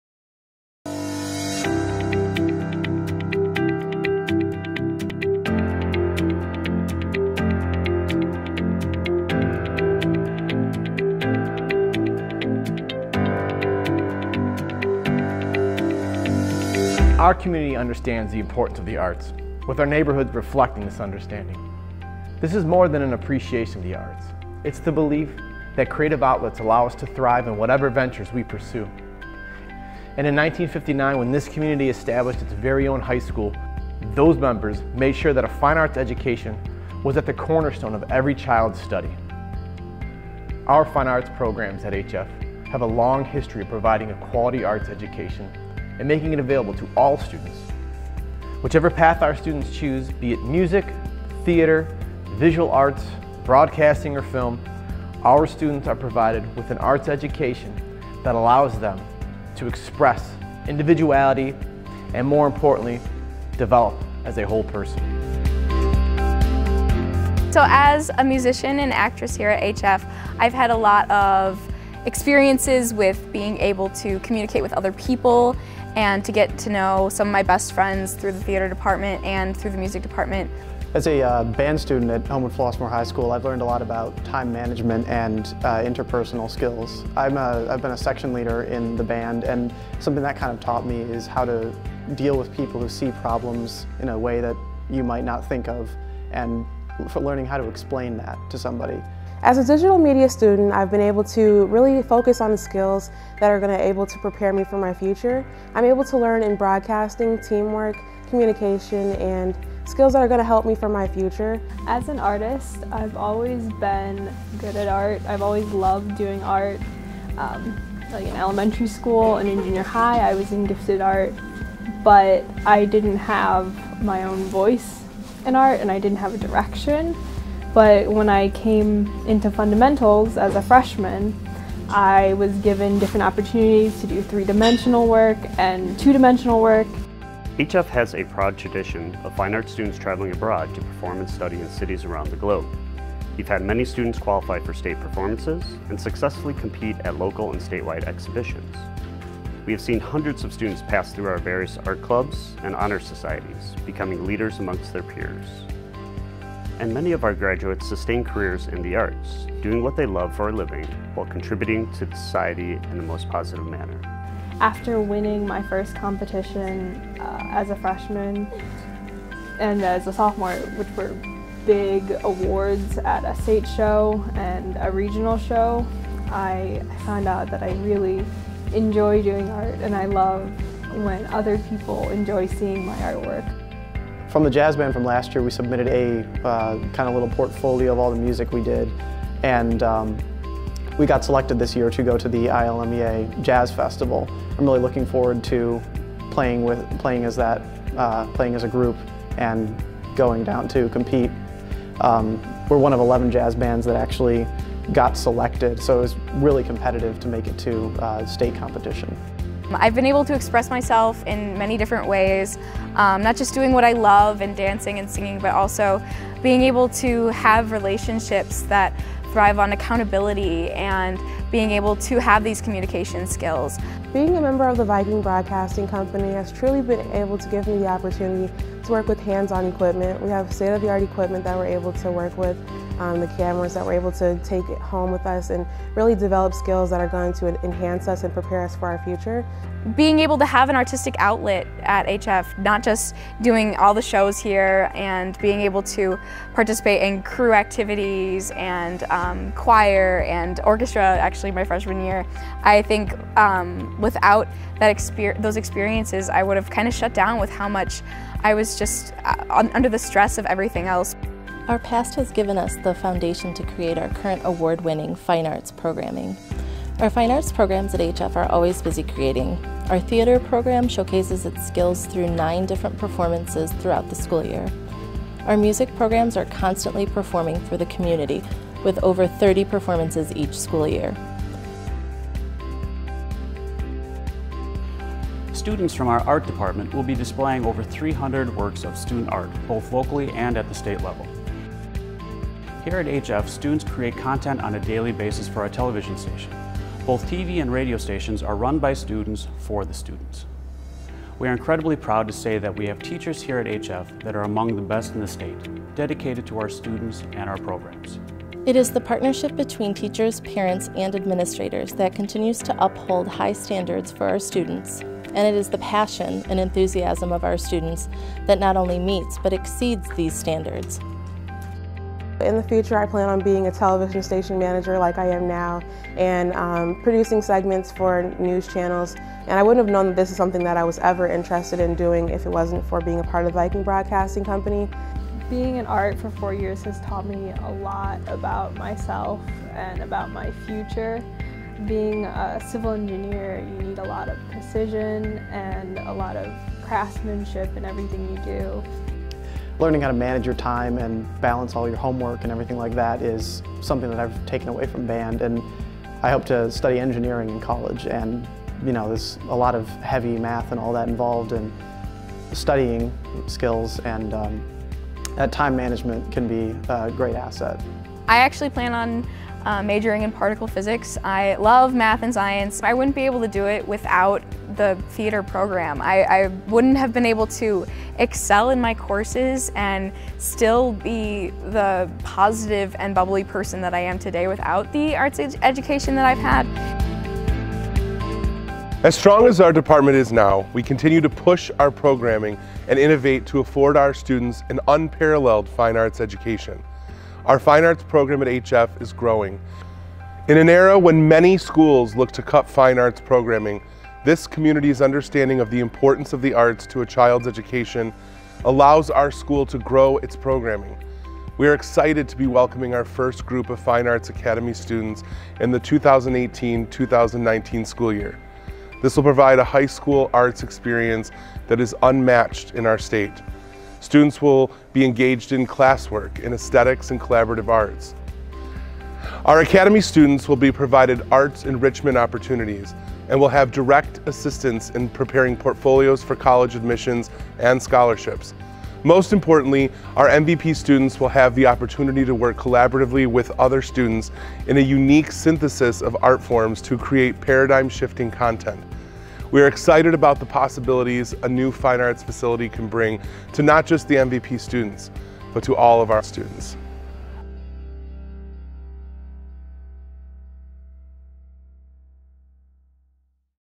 Fine Arts Facility Expansion
Learn more about the pending Fine Arts Facility Expansion at H-F High School via this video